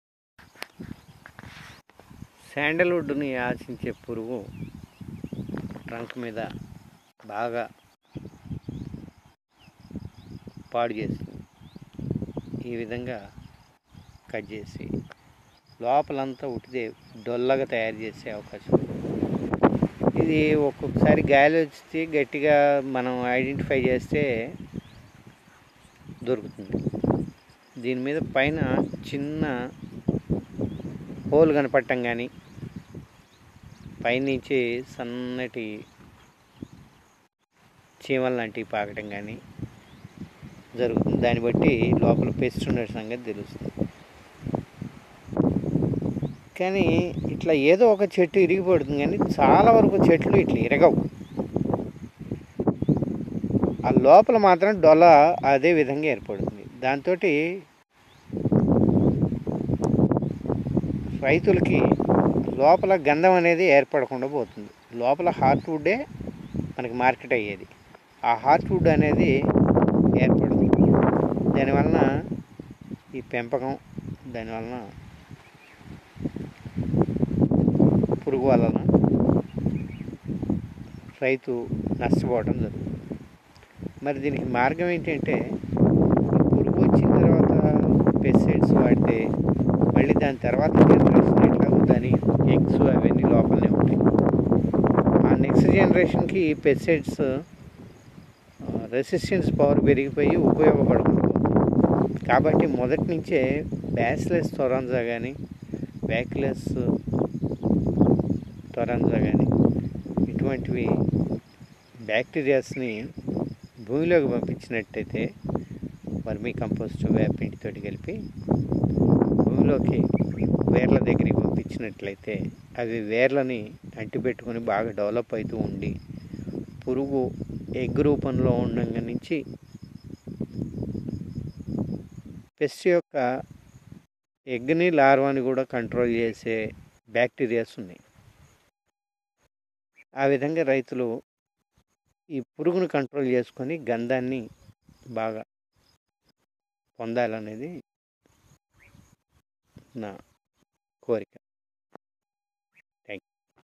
Sandalwood plants damage
Sandalwood plants damage with pest (stamborar) & control with bacteria contact no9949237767